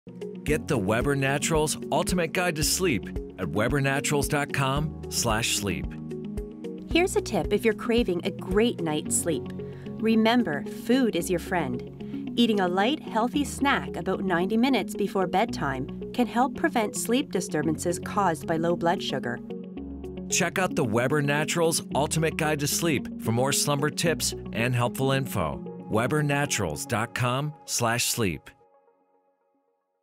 How to Sleep Better | Tip #1 Foods That Help You Sleep
Eating a light, healthy snack about 90 minutes before bedtime can help prevent sleep disturbances caused by low blood sugar.